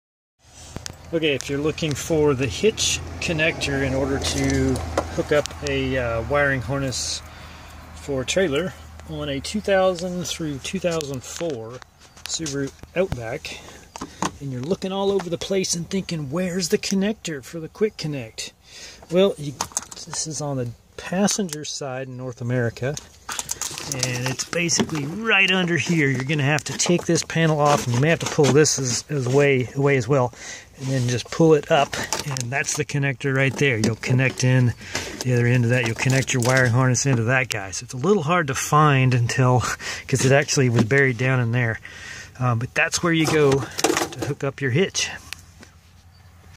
2nd Generation Subaru Outback - hitch wiring harness quick-connector location
This is where you install the CURT 55370 T-Connector in this generation Outback.